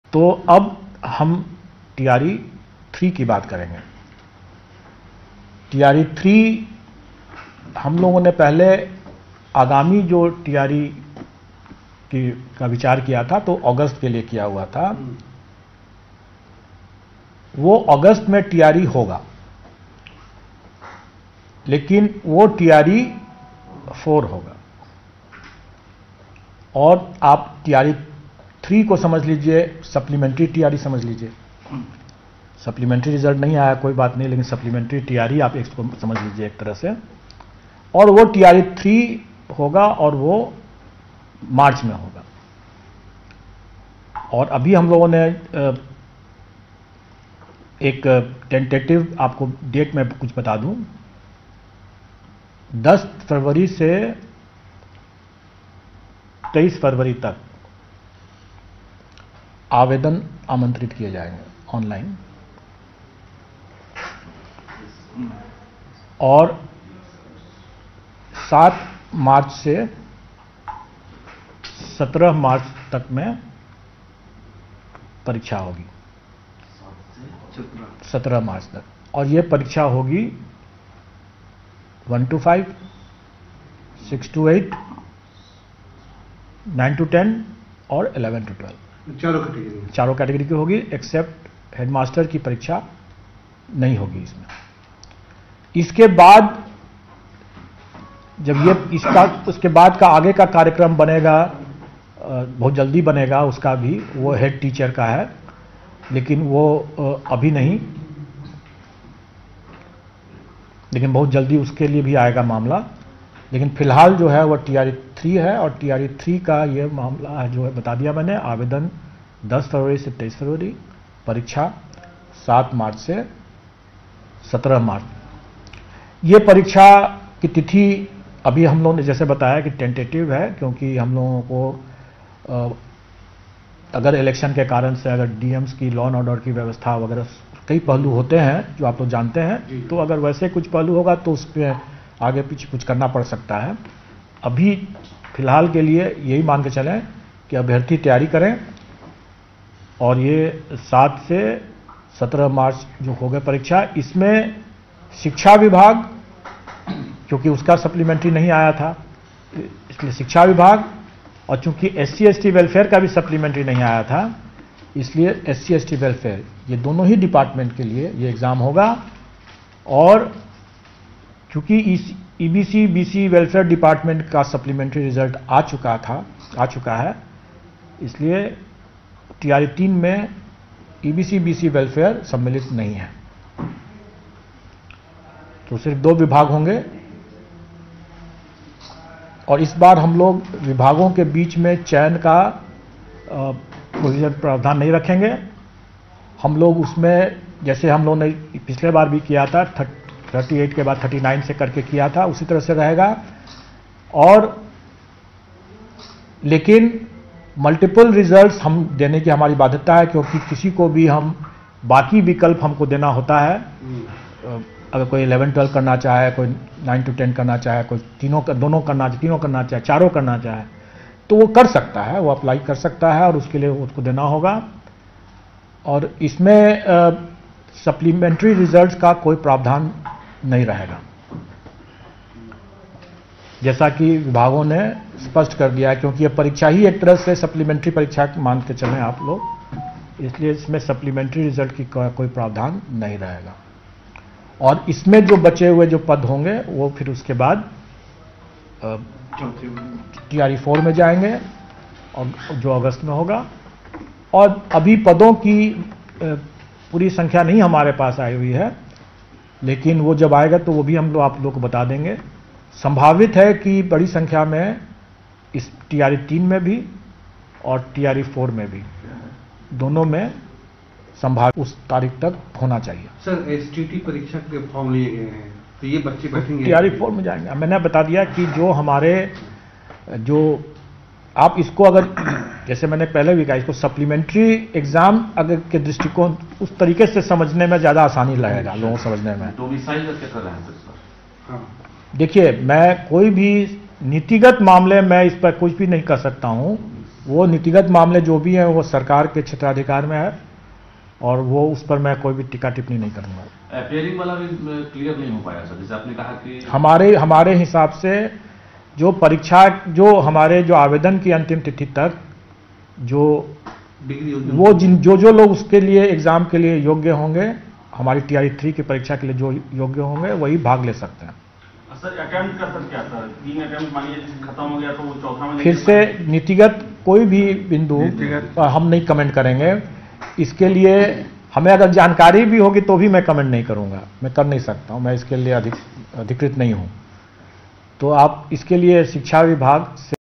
BPSC TRE.3 Online Application form and Exam Dates ||STET Appearing candidate is not allowed in TRE.3
●Online Application Form [10th Feb- 23rd Feb.2024]
●Tentative Exam dates- [7th March- 17th March 2024]
●STET Appearing candidates are not allowed in TRE.3